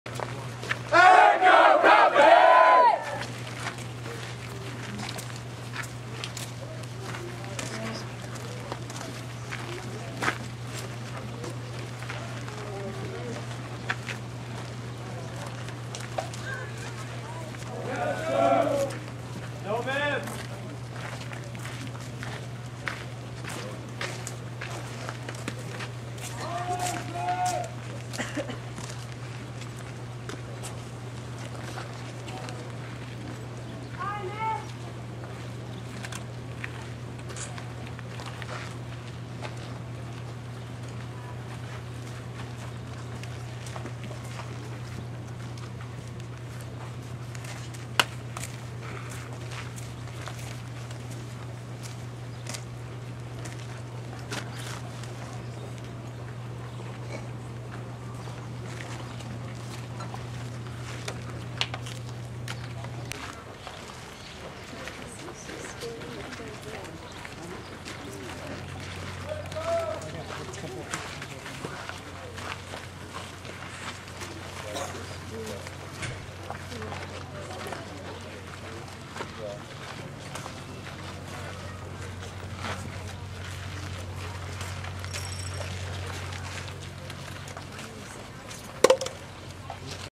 USCGA Class of 2016 preparing to march to flag ceremony
The US Coast Guard Academy class of 2016 at the Mystic Seaport in Mystic, CT preparing to march to their class flag ceremony.